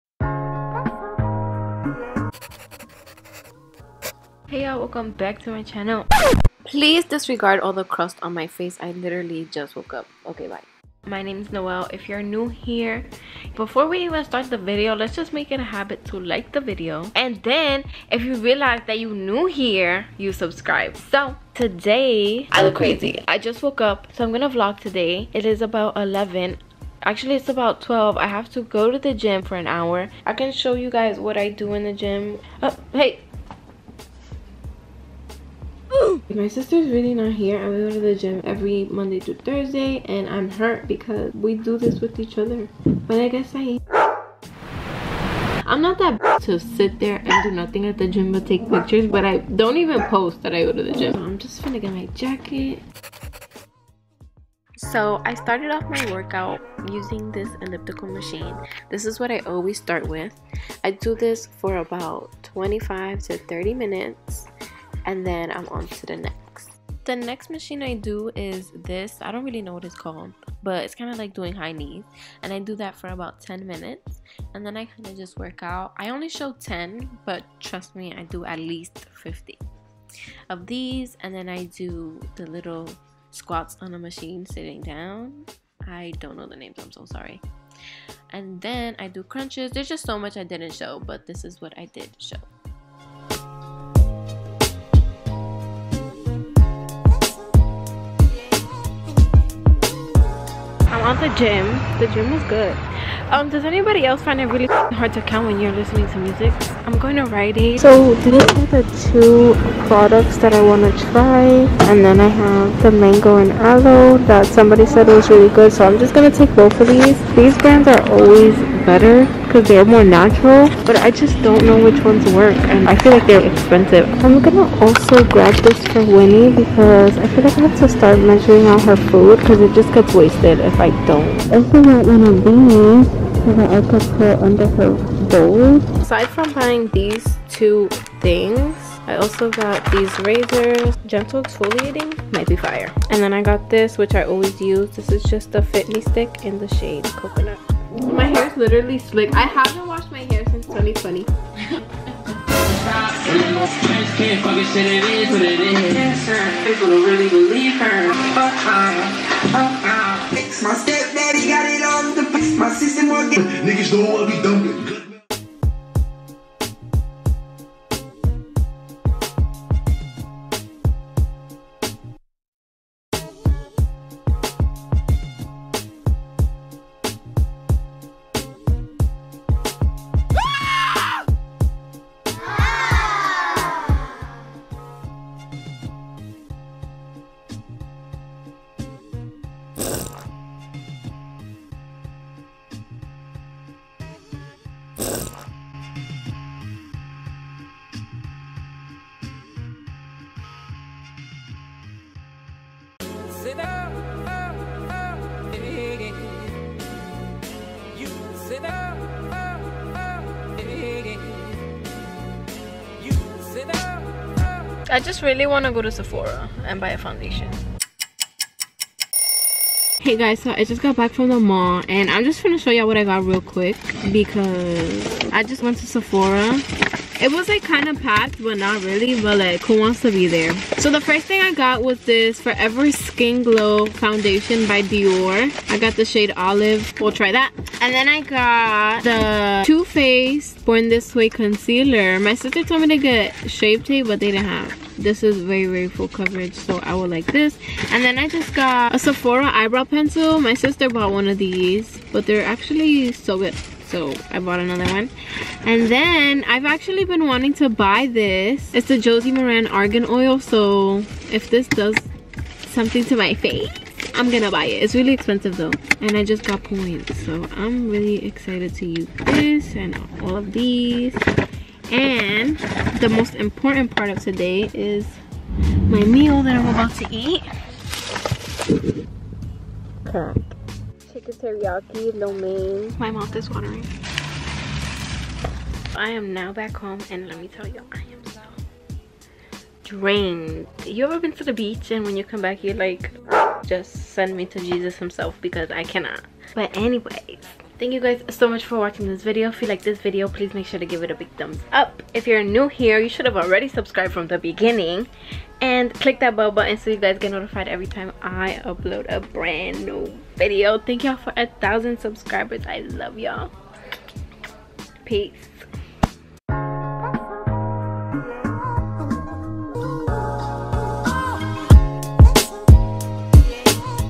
RUN ERRANDS WITH ME 2021🌸💜 gym + washday + Sephora haul 🤍
THANK YALL FOR WATCHING

**outro by  EVA GRACE**